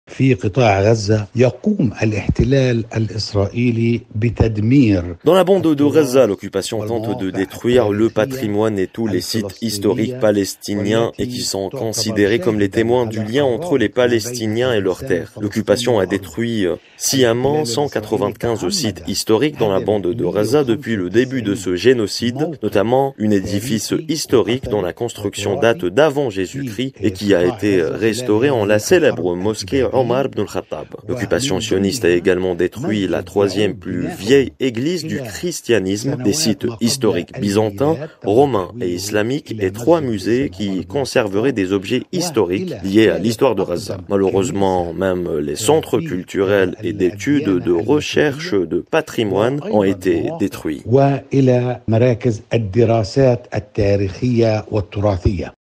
Info Internationale : Damtari Diliani - Analyste politique palestinien et membre du Fatah
L'analyste politique palestinien, Damtari Diliani, déplore aujourd'hui les attaques sionistes contre les symboles historiques palestiniens.